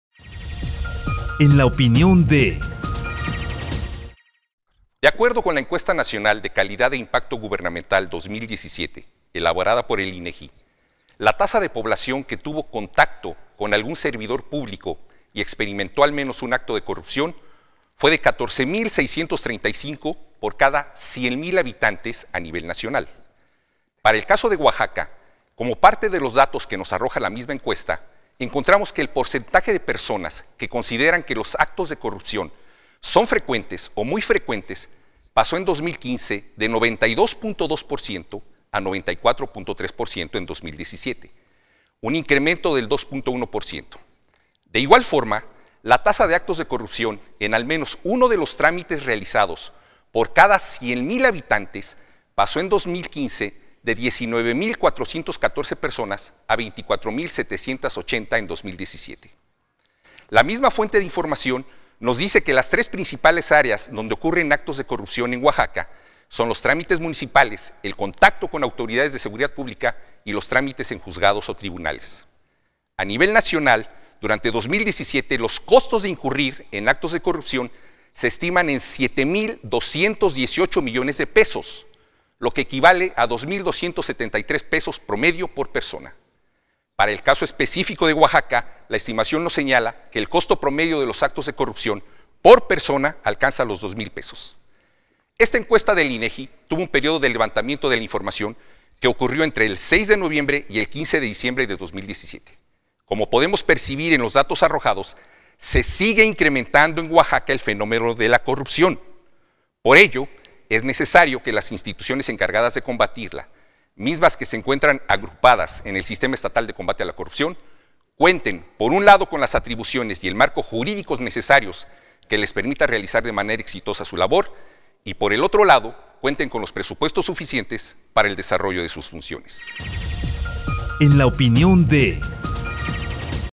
EN LA OPINIÓN DE: Pablo Munguía
Pablo Munguía
Integrante del Comité de Participación Ciudadana

Tema; La medición de la corrupción en Oaxaca.

De acuerdo con la Encuesta Nacional de Calidad e Impacto Gubernamental 2017 elaborada por el INEGI, la tasa de población que tuvo contacto con algún servidor público y experimentó al menos un acto de corrupción fue de 14 mil 635 por cada 100 mil habitantes a nivel nacional.